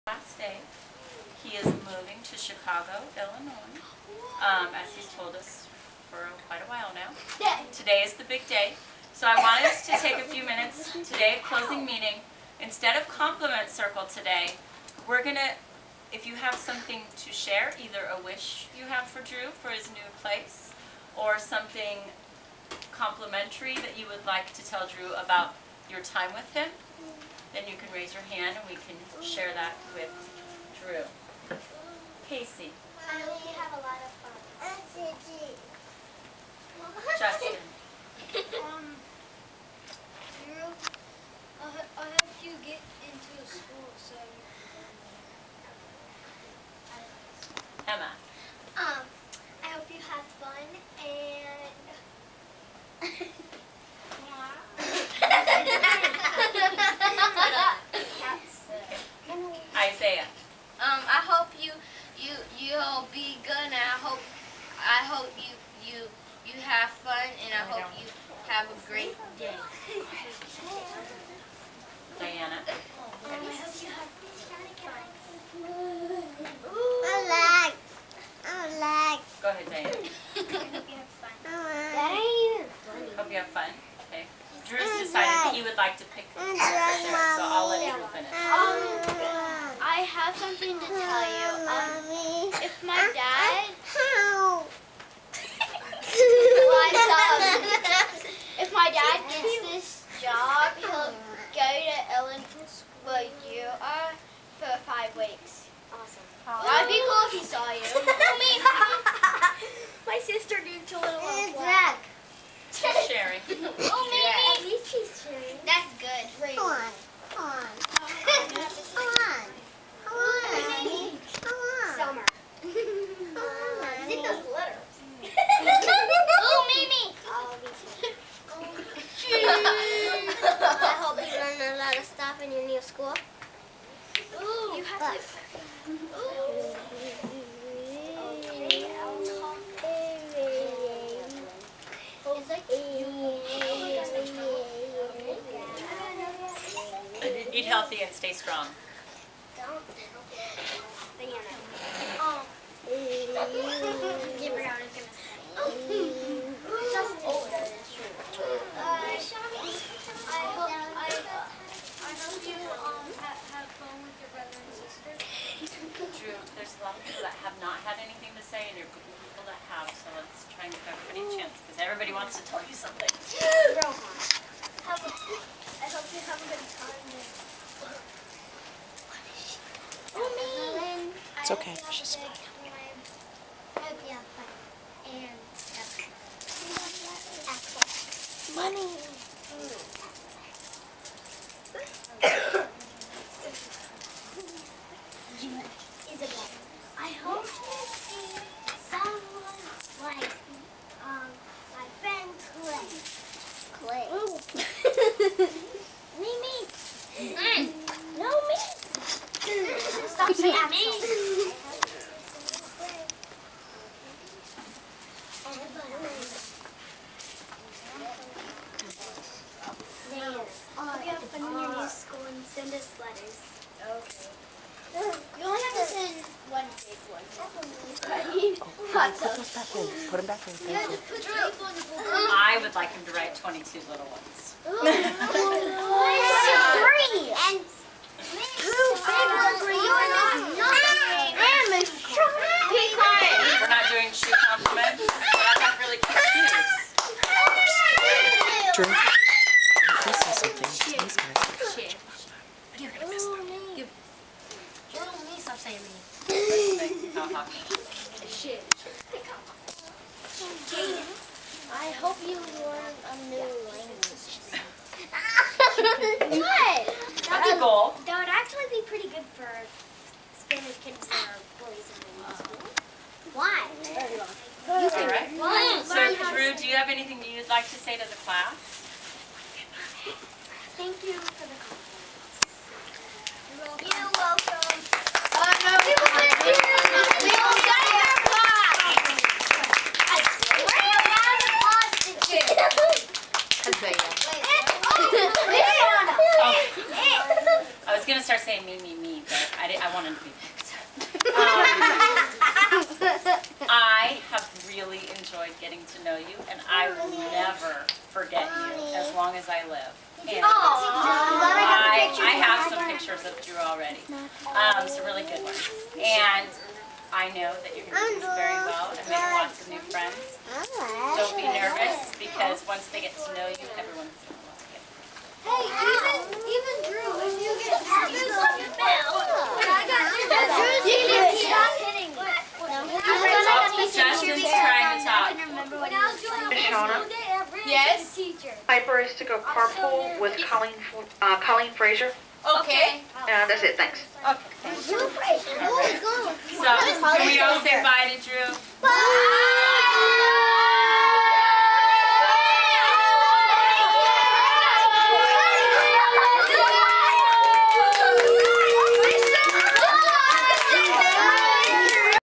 Saying goodbye
Dru's last day of school, Raleigh, March 2011